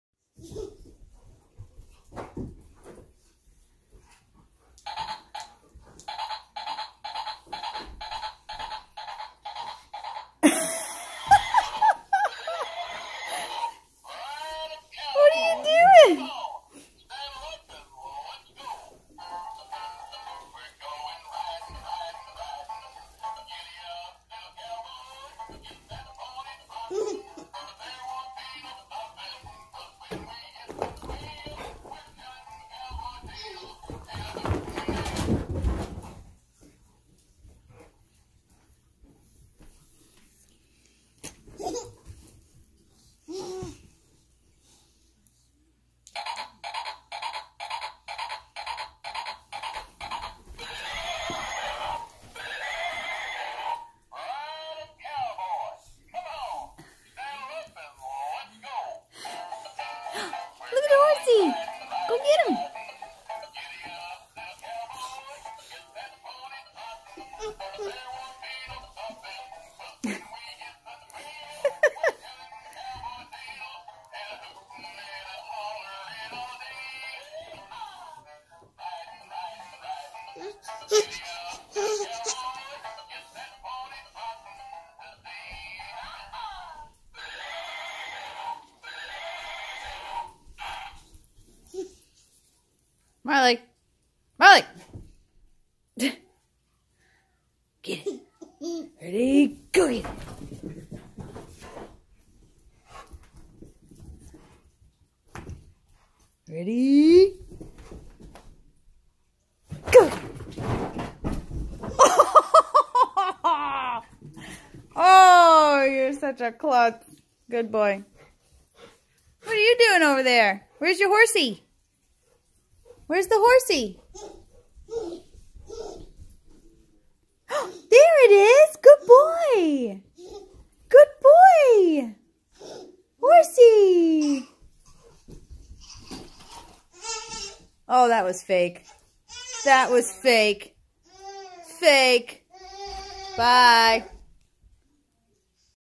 SHENANIGANS in the house
Messing around with Jackson and Marley. And Marley busted his a$$ lol